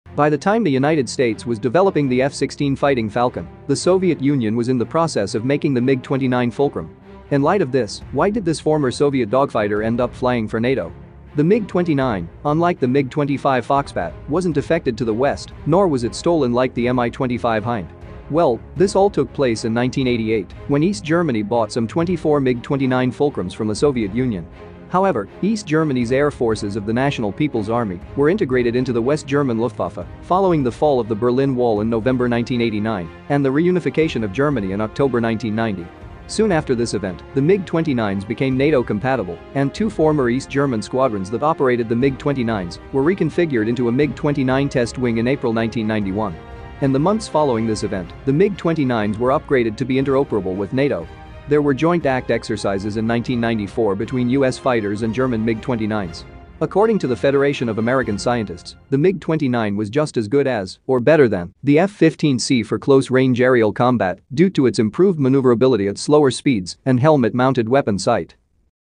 How Is The MiG-29 Became NATO Fighter? Was It Stolen?
By the time the United States was developing the F-16 Fighting Falcon, the Soviet Union was in the process of making the MiG-29 Fulcrum. In light of this, why did this former Soviet dogfighter end up flying for NATO? The MiG-29, unlike the MiG-25 Foxbat, wasn't defected to the West, nor was it stolen like the M.i-25 Hind. Well, this all took place in 1988, when East Germany bought some 24 MiG-29 Fulcrums from the Soviet Union.